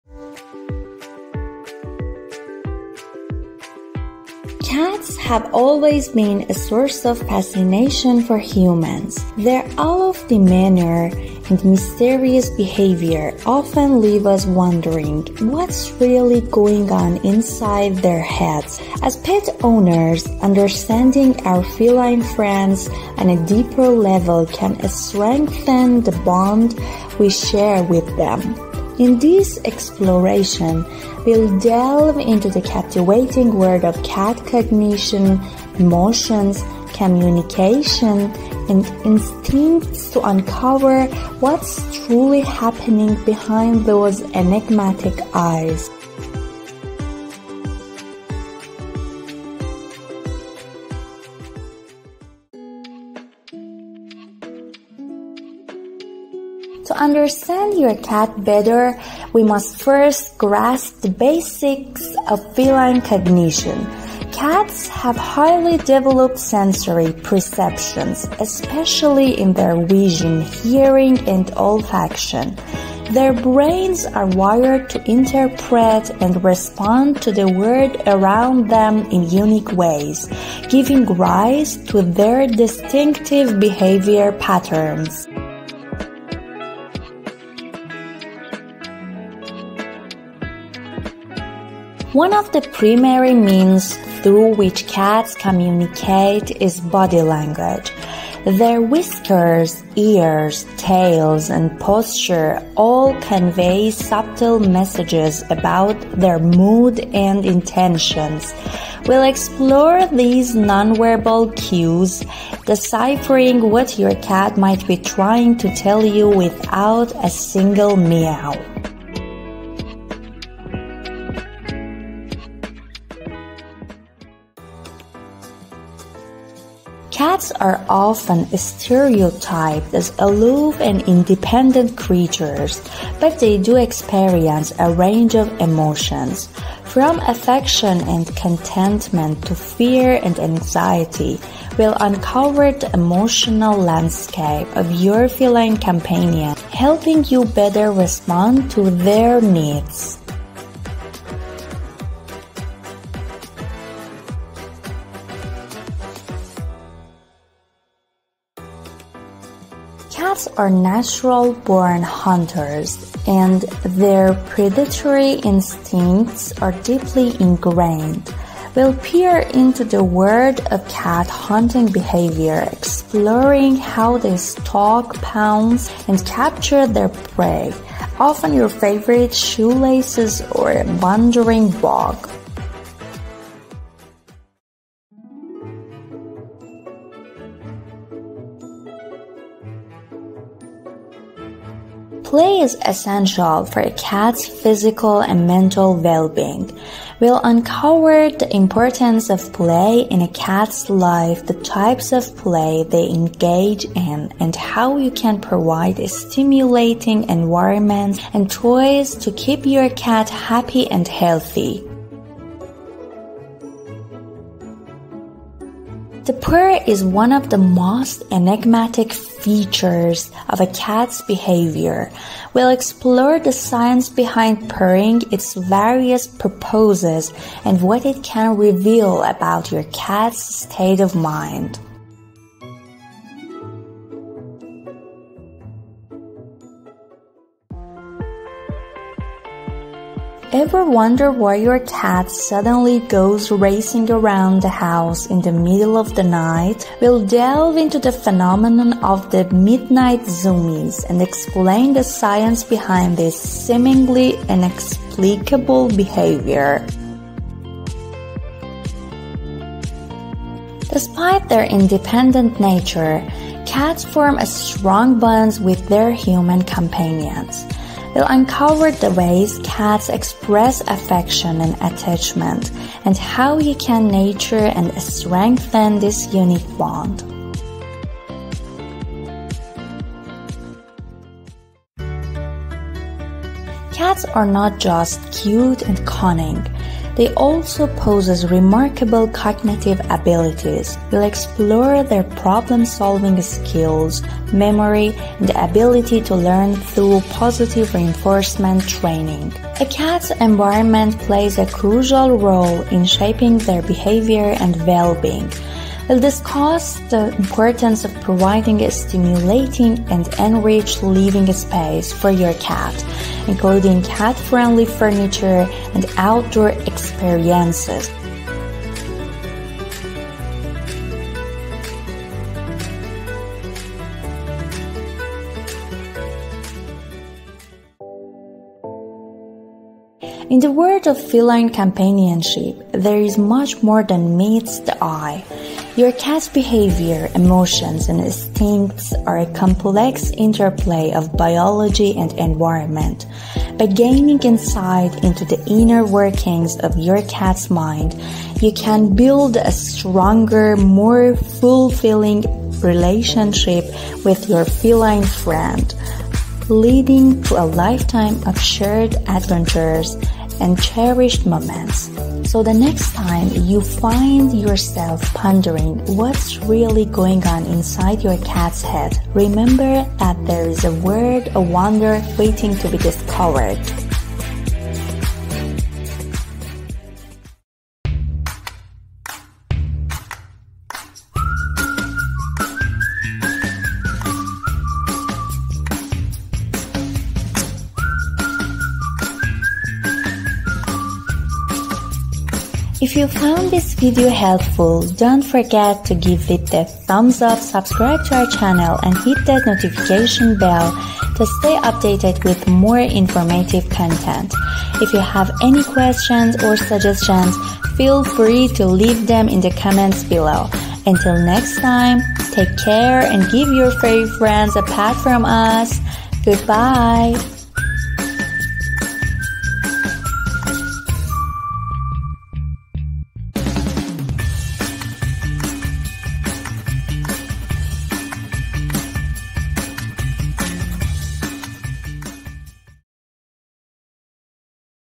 What's Really Going on Inside Your Cat's Head
Cats have always been a source of fascination for humans. Their aloof demeanor and mysterious behavior often leave us wondering: What's really going on inside their heads? As pet owners, understanding our feline friends on a deeper level can strengthen the bond we share with them. In this exploration, we'll delve into the captivating world of cat cognition, emotions, communication, and instincts to uncover what's truly happening behind those enigmatic eyes.